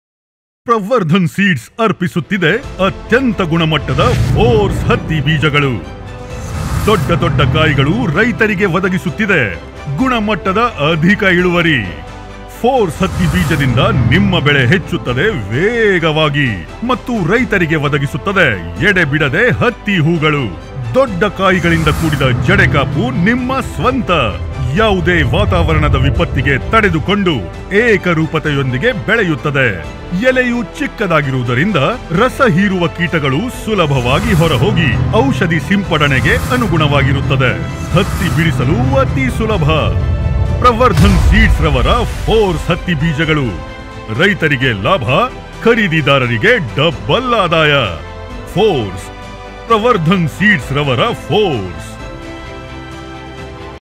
Force Cotton Hybrid || Kannada
Force Cotton Hybrid is high yielding open plant type with fast growth and strong stem. Force cotton hyrbid is having relatively big boll with easy picking and high yield. Force is Cotton hybrid of Pravardhan Seeds Private limited for South and Central Indian cotton markets.